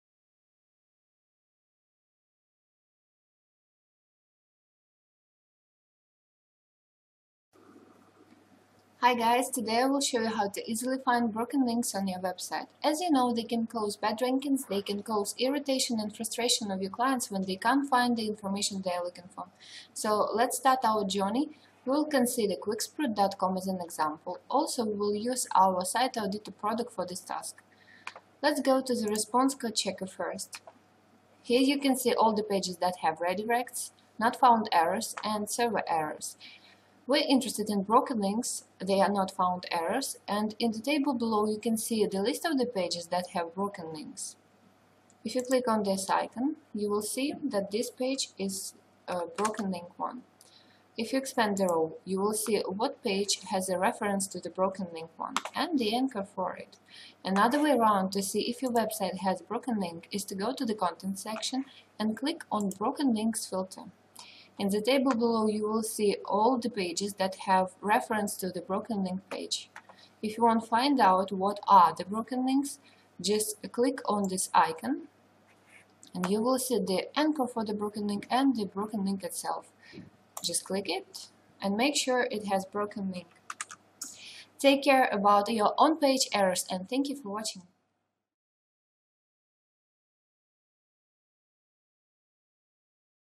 How to find broken links on your website. Broken links checker.
In this video we will show how you can easily find broken links on your website.

You can always try our product - Site Auditor for free.